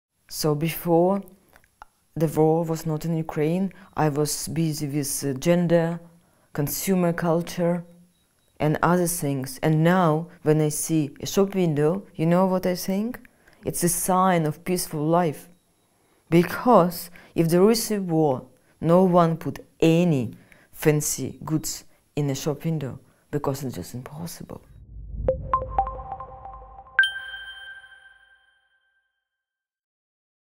Alevtina Kakhidze: Wie sich Kunst in Zeiten des Krieges verändert (Teaser)
Die ukrainische Künstlerin Alevtina Kakhidze hat erst mit dem Krieg begonnen, politische Kunst zu machen. Im Interview (in englischer Sprache) erzählt sie, wie sich ihre Haltung zur Kunst verändert hat.